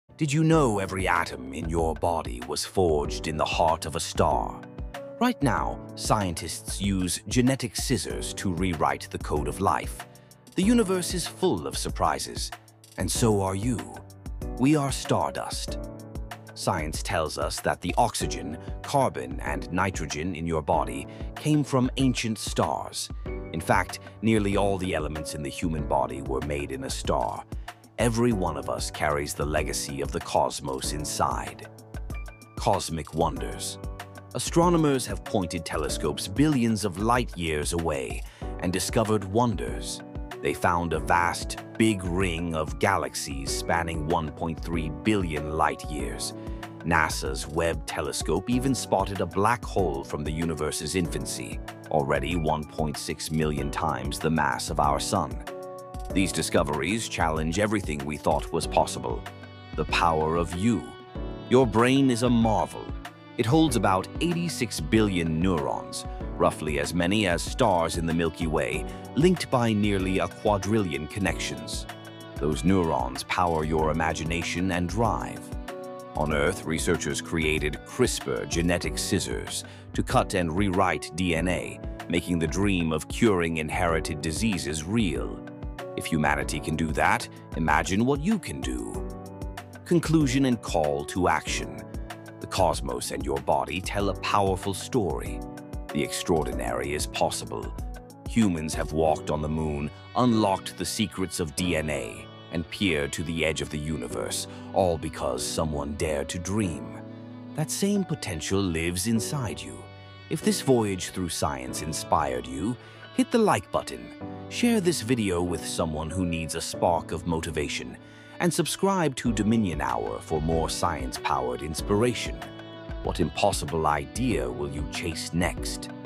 You Are Made of Stars – The Science That Will Change How You See Yourself 💥🚀
What if I told you that every atom in your body was born in a star? 🌟
This 3-minute animated short reveals the mind-blowing science behind your true potential – from cosmic dust to genetic miracles like CRISPR. This isn’t just motivation… it’s truth backed by science.

👨‍🚀 Explore how the universe lives within you
🧬 Learn how science is rewriting the limits of life
🔥 Get inspired to chase the “impossible”

👇 COMMENT what "impossible" goal you're chasing!
👍 LIKE if this video gave you chills